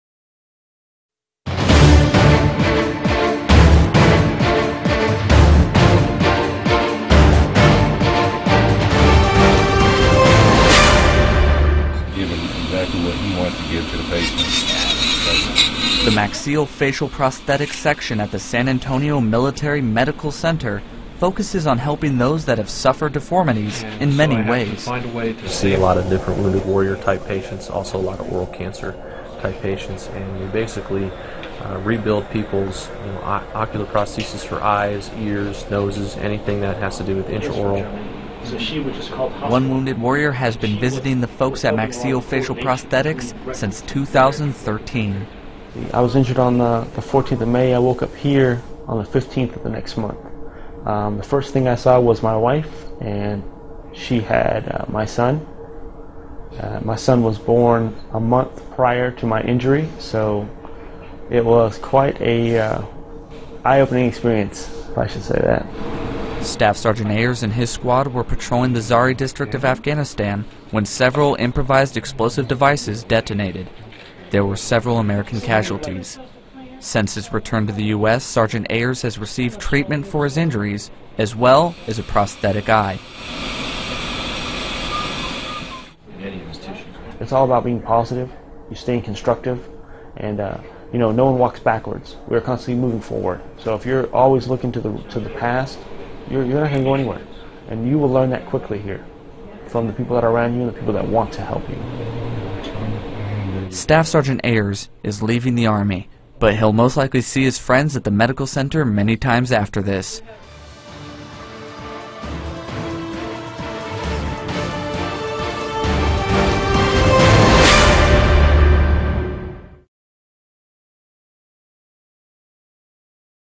Soldiers Update: Ayers' Eye
Staff Sgt. Anthony Ayers lost an eye when an IED exploded during a patrol in Afghanistan. He received a new eye from the Maxillofacial Prosthetics specialists at Fort Sam Houston, TX.